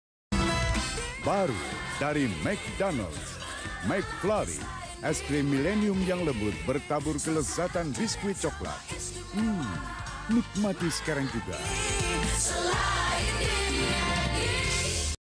Iklan Mc Donald's - Mc Flurry (1999) @ RCTI
Pernah Ditayangkan Saat Pemutaran Tuyul & Mbak Yul & Jinny Oh Jinny